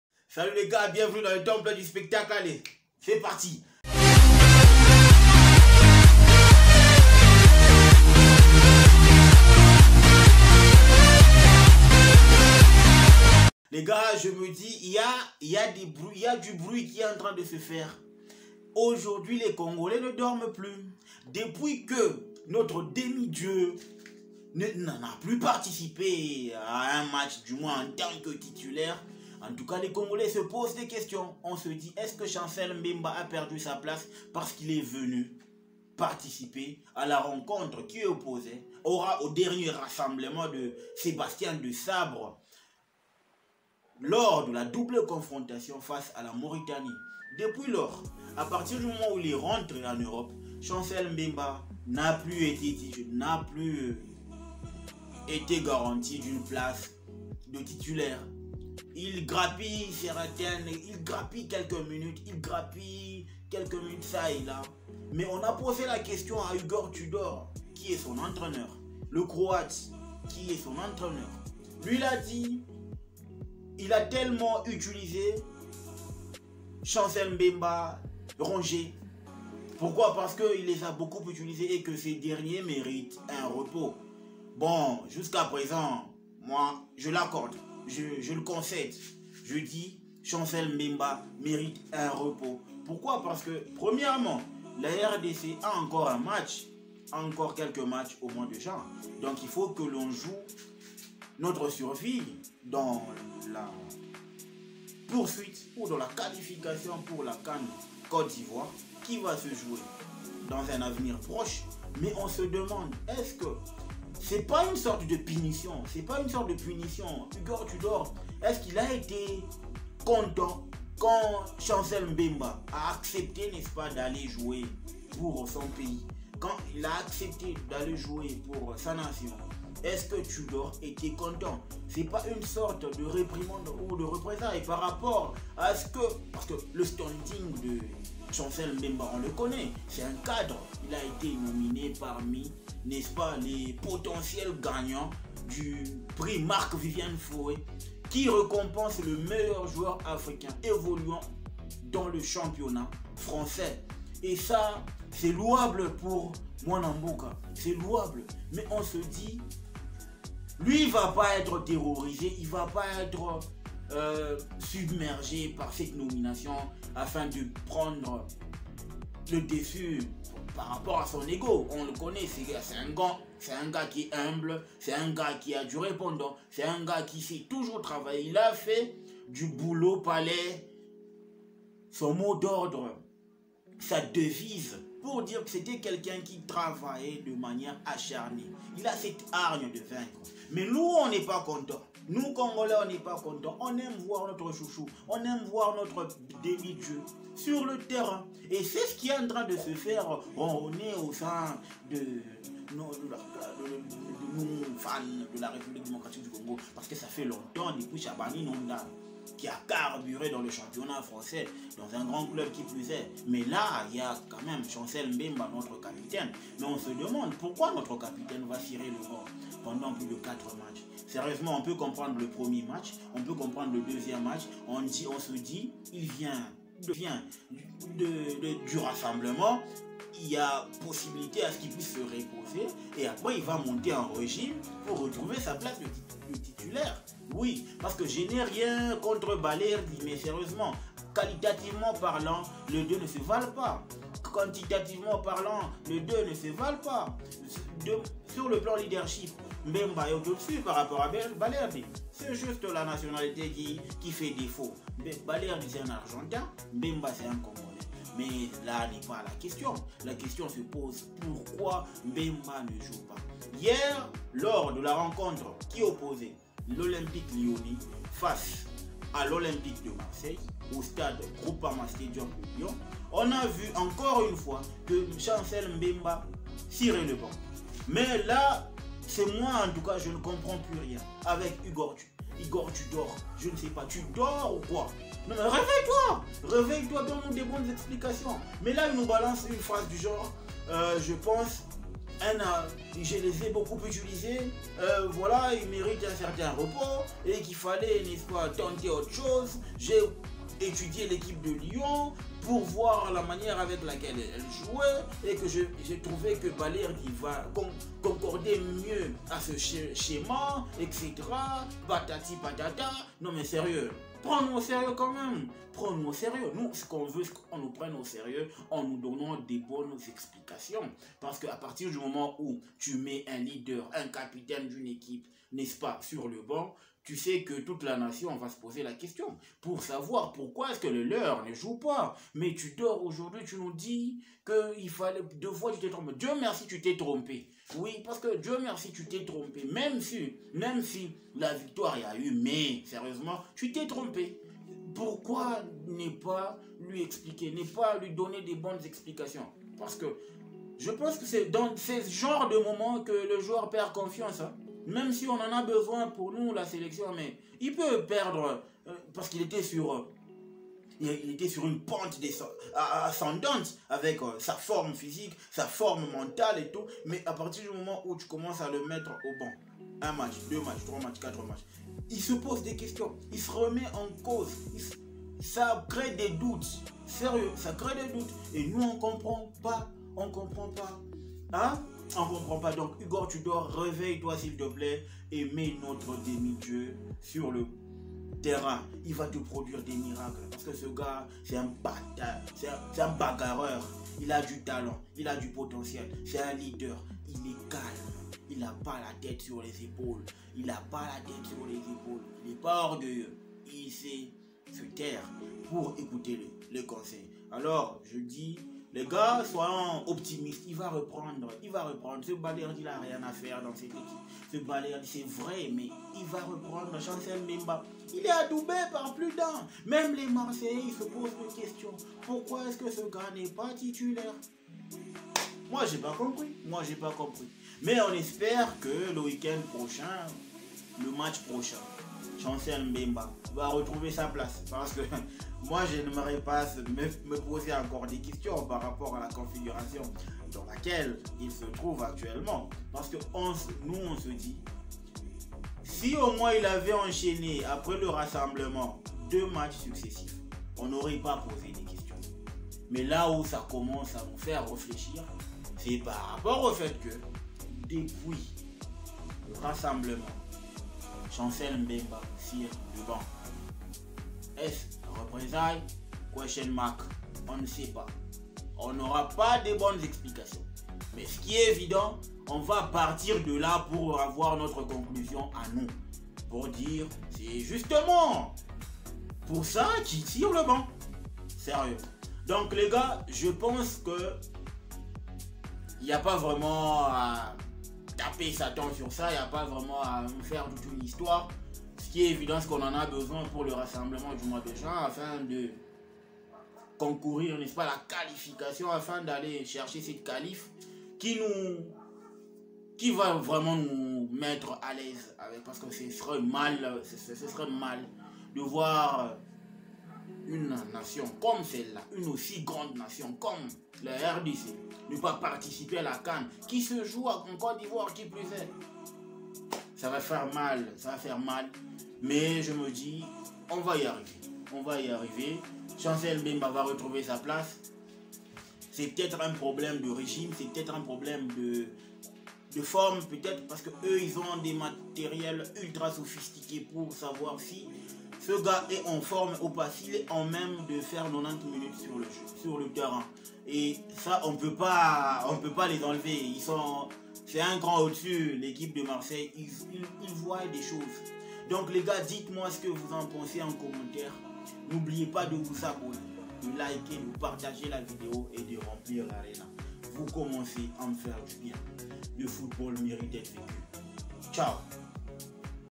POURQUOI CHANCEL MBEMBA N'EST PLUS TITULAIRE À L'OM ?
depuis qu'il a participé au dernier rassemblement avec les léopards mbemba a perdu sa place de titulaire